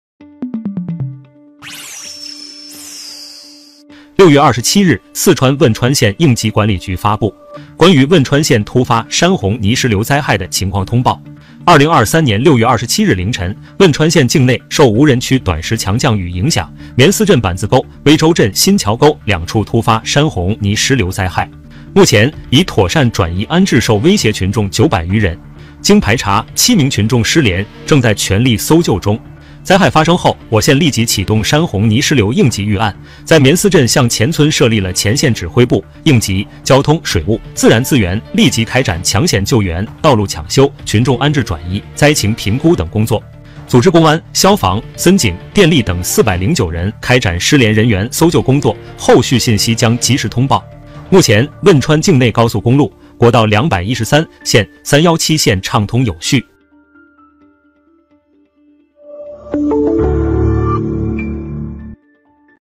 四川汶川突发山洪泥石流：7名群众失联，正全力搜救中
四川汶川突发山洪泥石流：7名群众失联，正全力搜救中A sudden flash flood and mudslide in Wenchuan, Sichuan: 7 people lost contact and are fully searching and rescuing